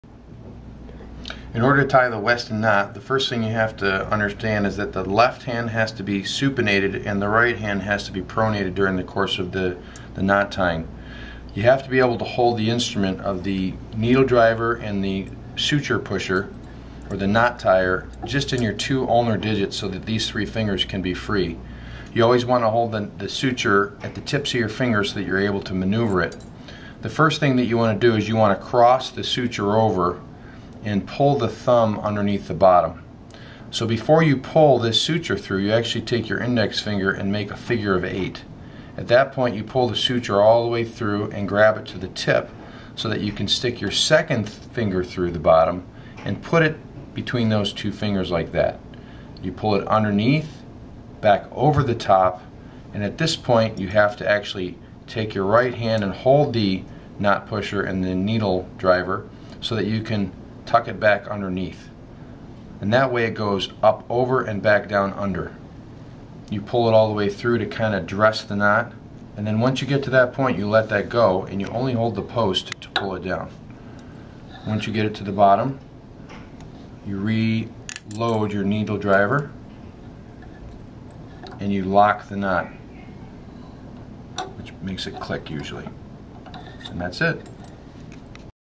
Dr. Marc Milia Westin Knot
Instructional video for WSU residents in training to learn "the secret" behind this arthroscopic knot